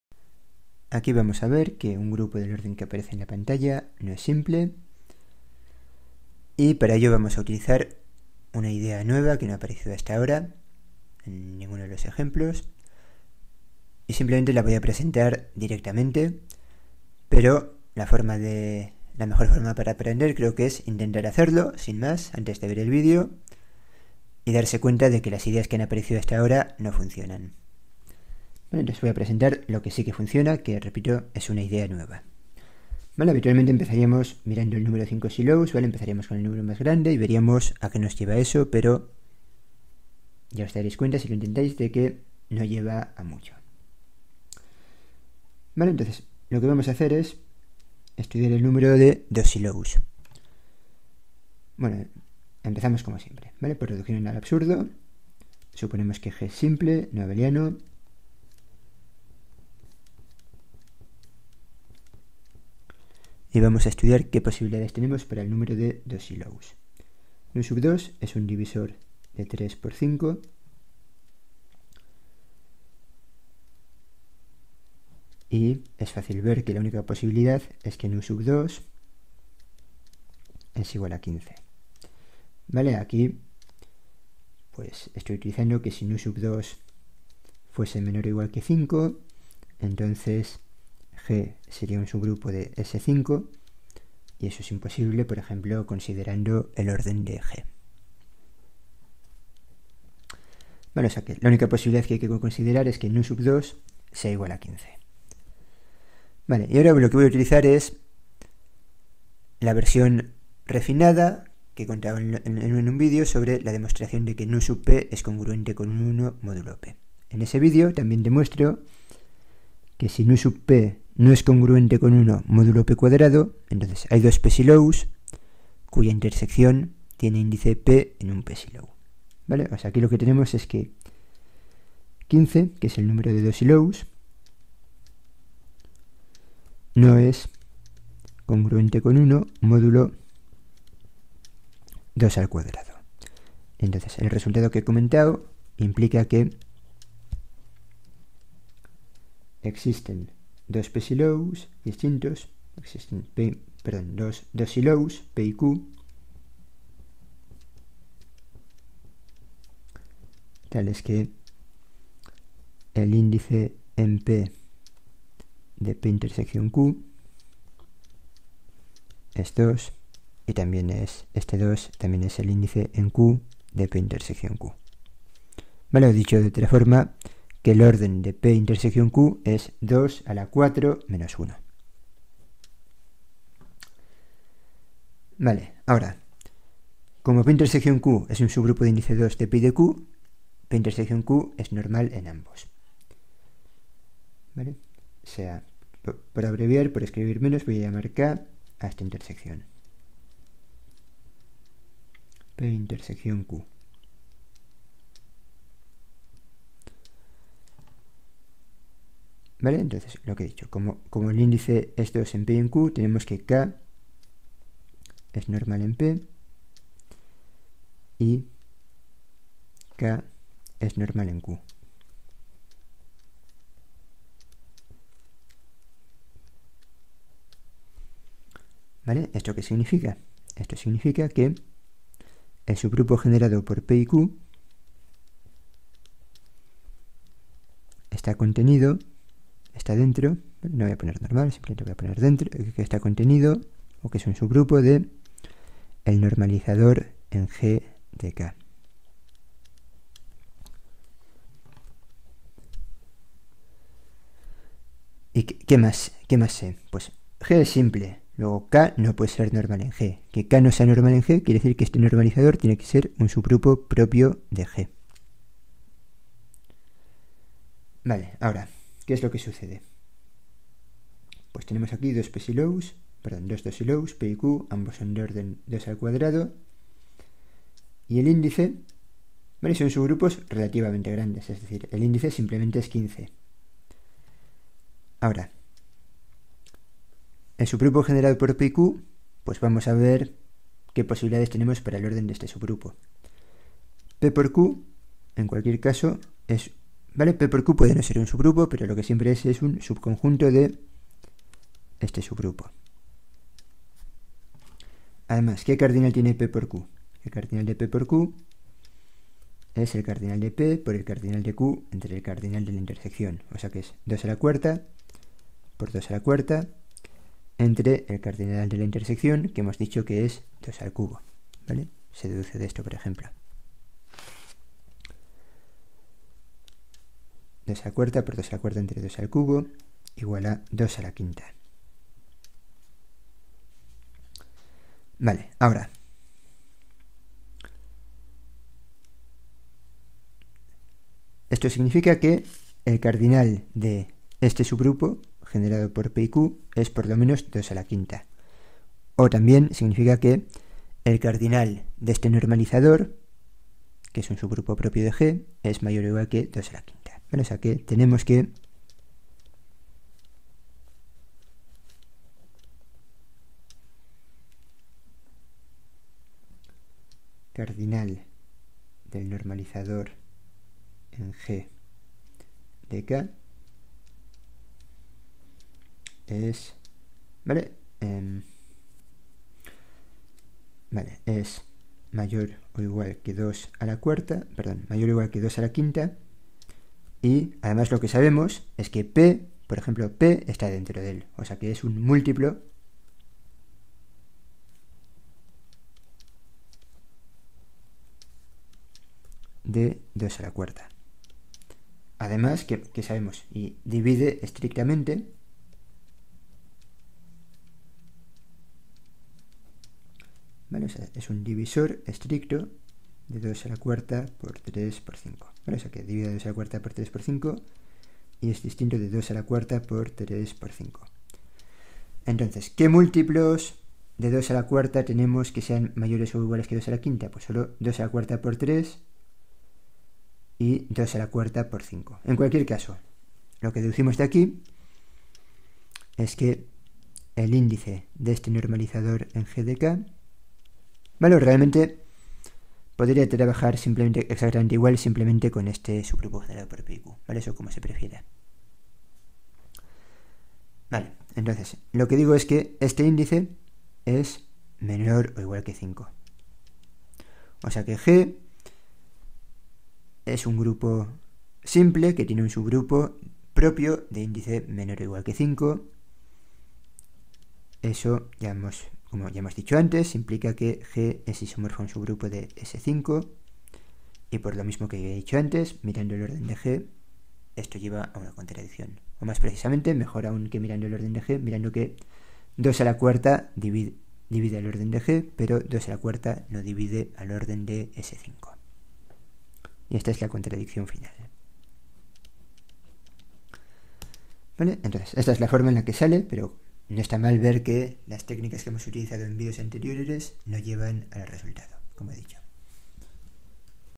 No hay grupos simples de orden 240
Demostramos que no hay grupos simples de orden 240. Utilizaremos el refinamiento sobre el resultado de la congruencia para el número de p-Sylows visto en un vídeo anterior.